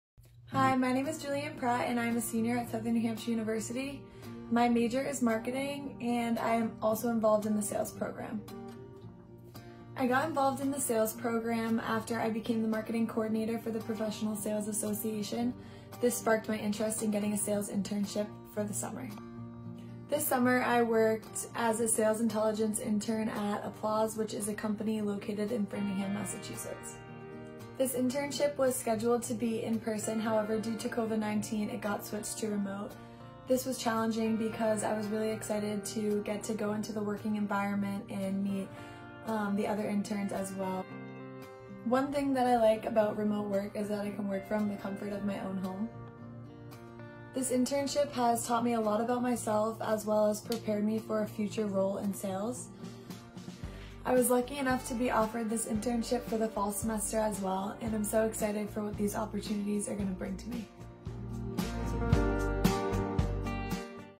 Internship Furthers Senior’s Desire for Marketing Career
Senior Julianne Pratt used her classes and participation in the Professional Sales Association to land a great internship with Applause, a digital crowdtesting firm based in Framingham, MA.